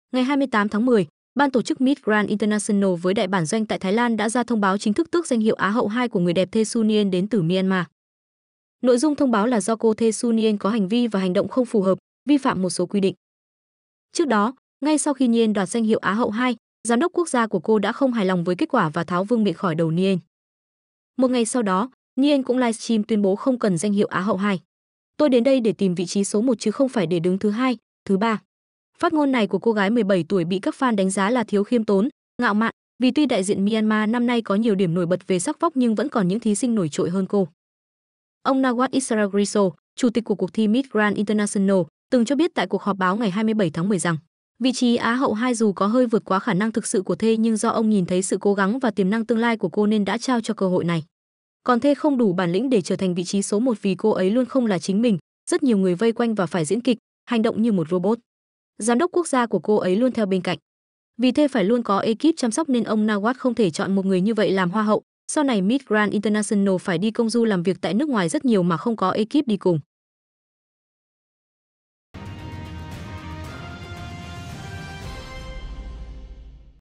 Hoa hậu Hòa bình Quốc tế 2024: Á hậu 2 bị tước vương miện
Ngày 28/10, Ban tổ chức Miss Grand International với "đại bản doanh" tại Thái Lan đã ra thông báo chính thức tước danh hiệu Á hậu 2 của người đẹp Thae Su Nyein đến từ Myanmar.

Nội dung thông báo là do "cô Thae Su Nyein có hành vi và hành động không phù hợp, vi phạm một số quy định".